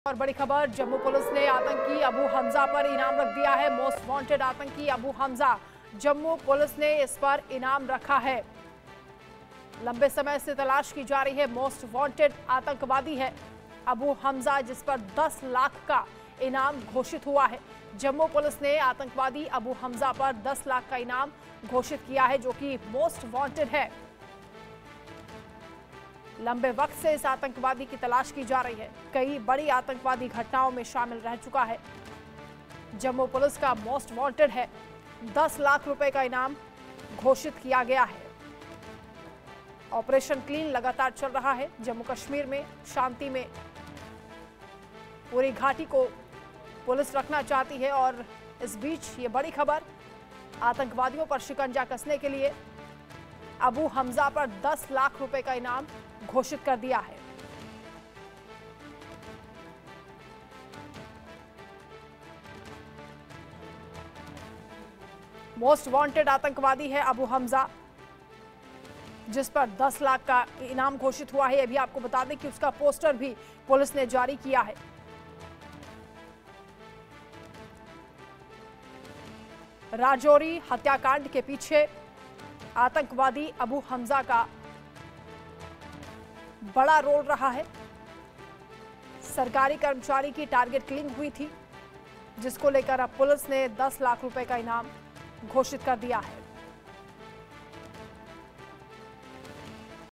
Jammu Kashmir Terrorist Attack: जम्मू पुलिस ने आतंकी अबू हमजा पर रखा 10 लाख का इनाम...| India News
लोकसभा चुनाव की सरगर्मी के बीच सोमवार (22 अप्रैल) को जम्मू के राजौरी जिले में कुछ अज्ञात हमलावरों ने एक शख्स की गोली मारकर हत्या कर दी थी. पुलिस ने इस हमले के पीछे एक विदेशी आतंकी अबू हमजा का हाथ बताया है. इतना ही नहीं पुलिस ने इस आतंकी के बारे में जानकारी देने वाले को 10 लाख रुपए इनाम की घोषणा की है. जम्मू कश्मीर पुलिस ने उसे वांटेड आतंकवादी बताया है.

On Monday (April 22), amidst the excitement of Lok Sabha elections, a person was shot dead by some unknown assailants in Rajouri district of Jammu. Police have blamed a foreign terrorist Abu Hamza for being behind this attack. Not only this, the police has announced a reward of Rs 10 lakh for anyone giving information about this terrorist. Jammu and Kashmir Police has described him as a wanted terrorist.